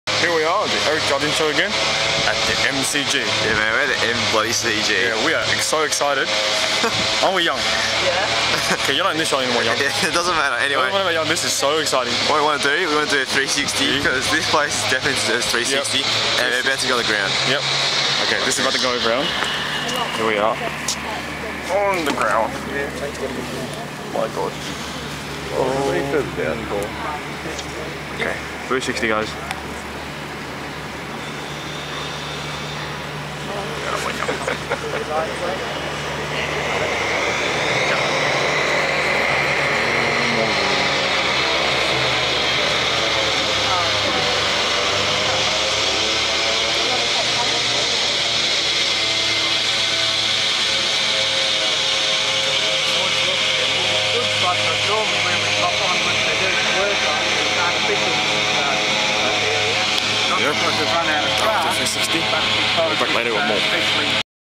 MCG 1: Eric and Jia Lin Show
WOOT WOOT!!! The MCG, the sporting mecca of Australia. Filmed it as we we're walking onto the grass, quite an unforgettable moment.